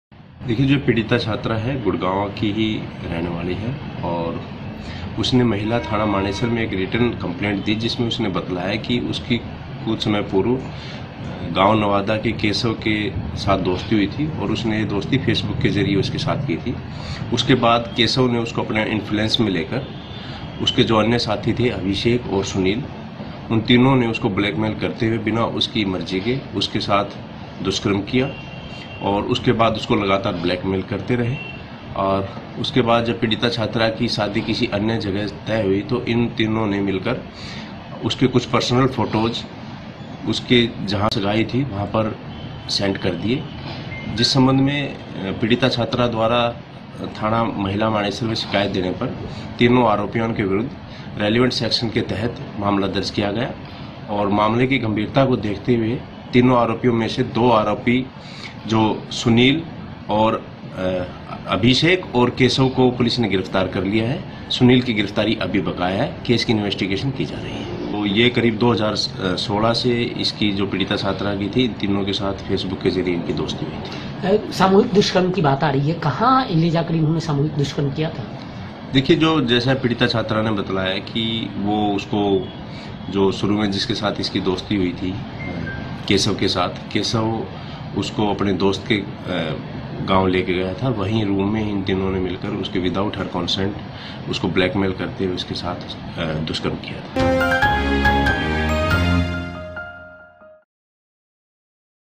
Woman allegedly gang raped by her ‘Facebook’ friends in Gurugram
A 23-year-old woman was allegedly raped by three of her friends in Gurugram on Saturday. “The girl met one of the boys through social media and after few meetings her friend along with two others raped her, said Gurugram PRO Ravinder Kumar”.